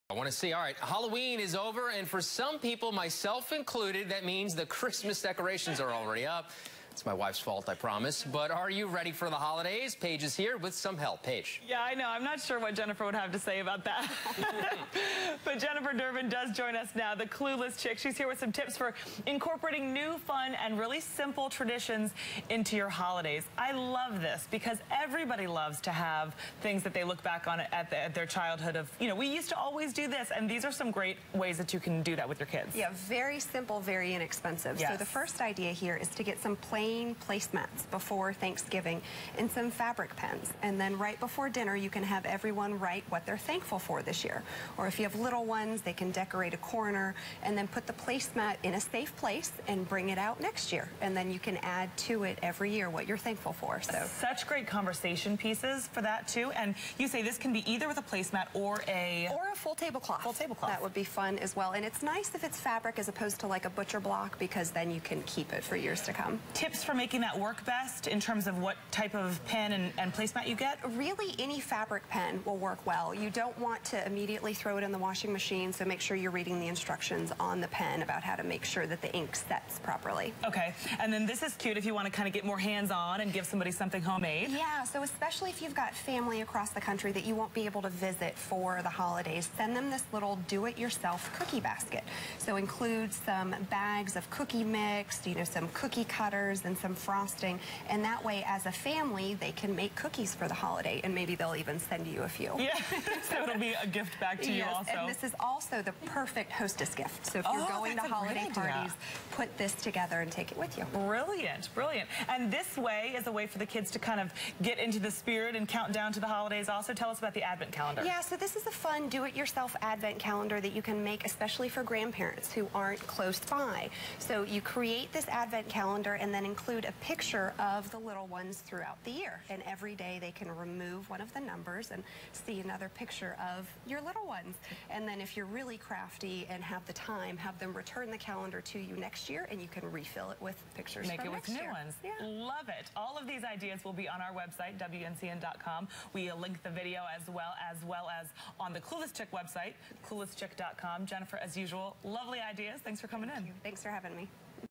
Tips on starting fun, new holiday traditions
Jennifer Durbin, author of Party Planning Tips for the Clueless Chick gives us some great tips for incorporating a few fun, and of course simple, new traditions into your holidays.